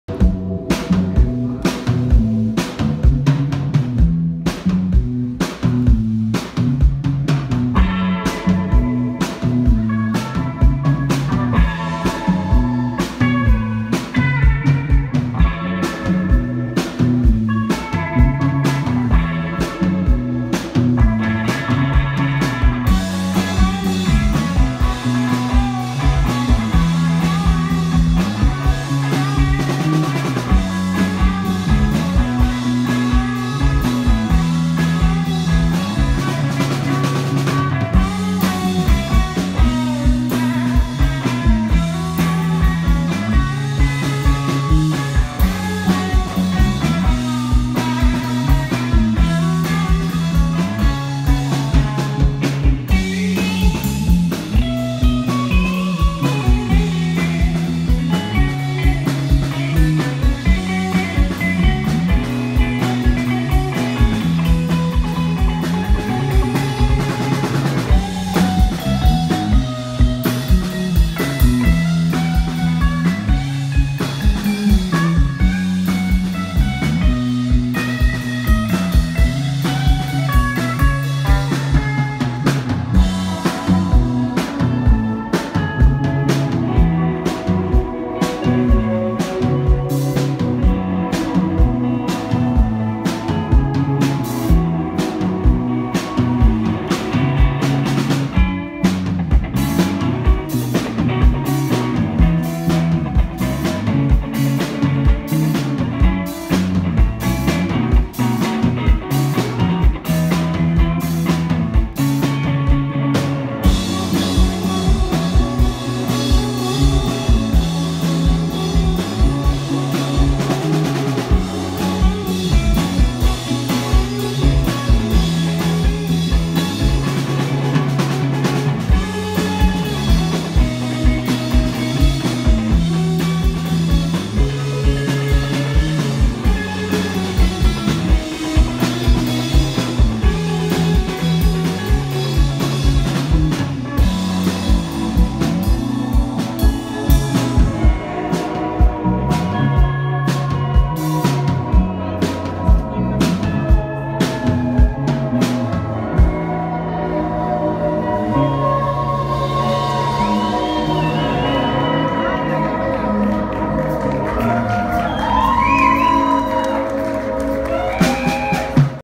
Popload no Picnik Festival - Supervibe
Popload em Brasília. Performance da banda local Supervibe, do Gama, no festival do Farol da Torre de TV. junho/2017